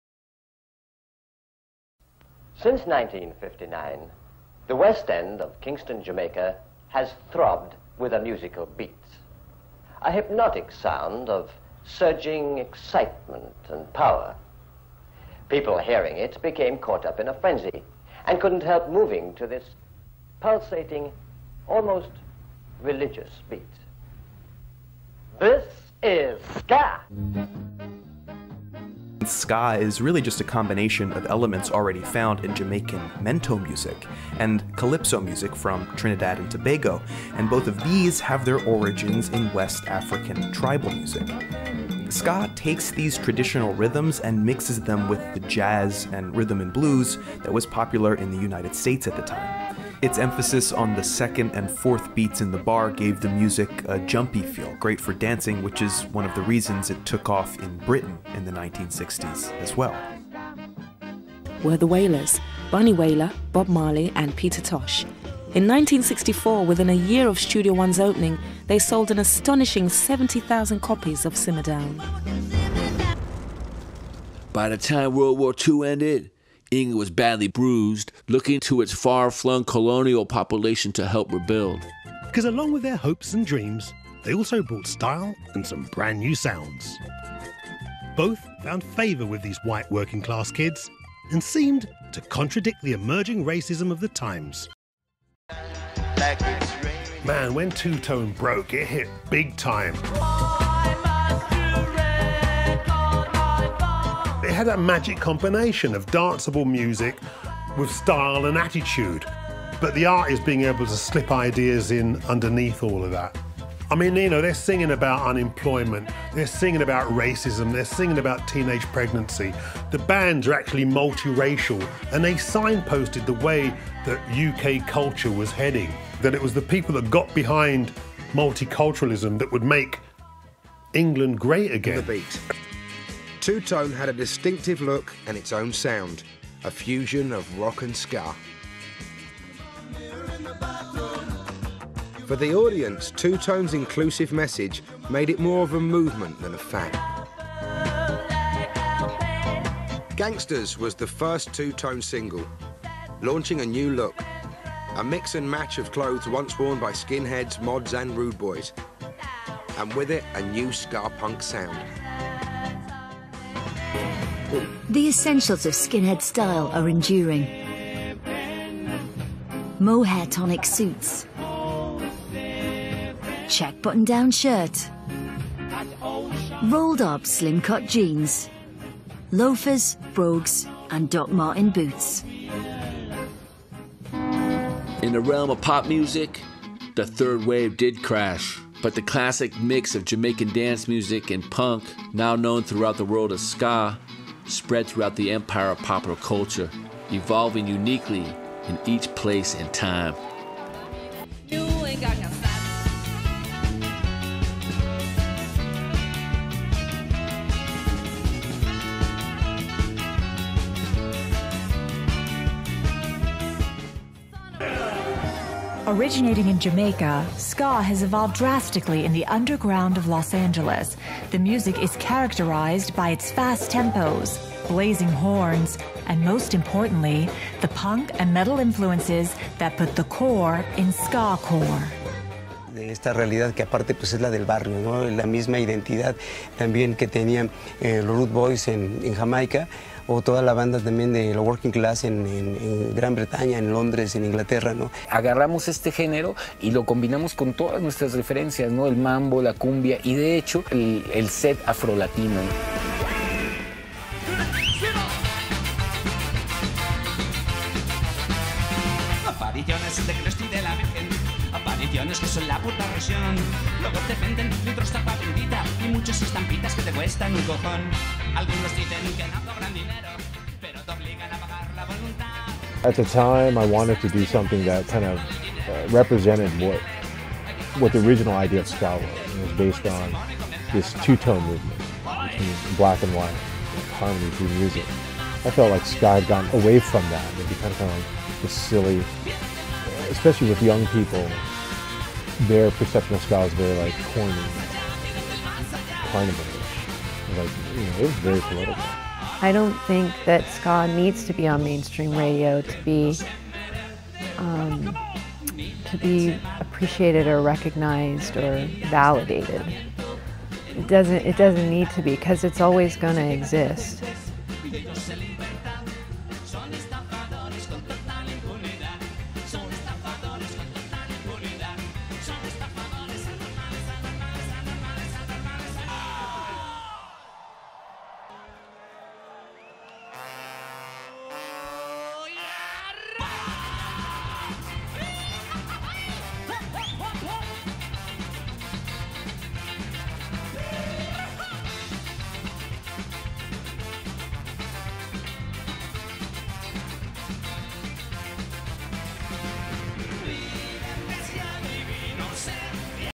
ska'd for life Desktop Documentary
Desktop documentary explaining history of ska